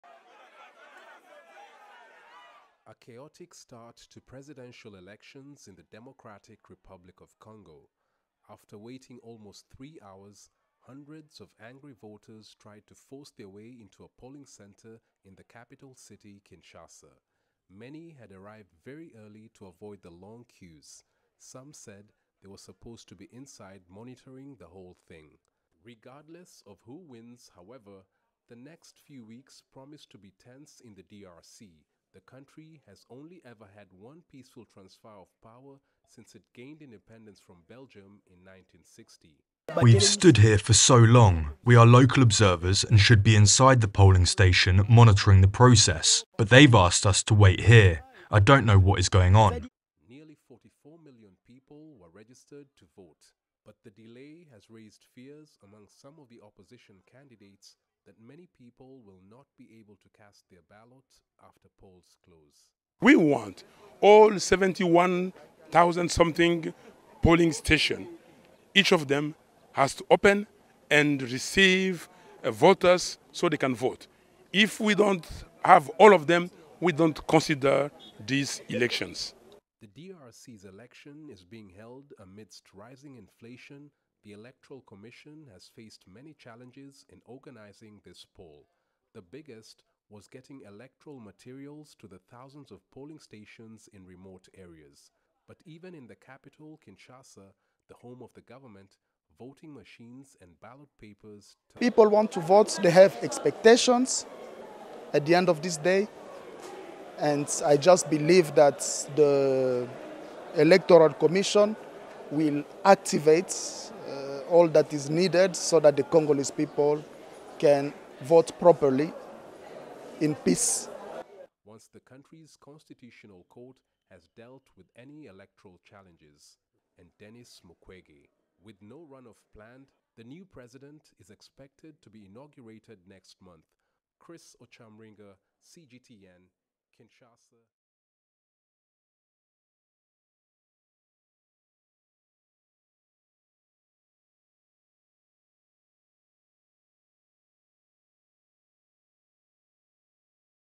DR Congo Holds General Elections Amid Insecurity, Logistics Problems
Nearly 44 million voters in the Democratic Republic of Congo presidential election eventually got going in many polling stations across the country on Wednesday, after long delays in receiving the necessary electoral materials.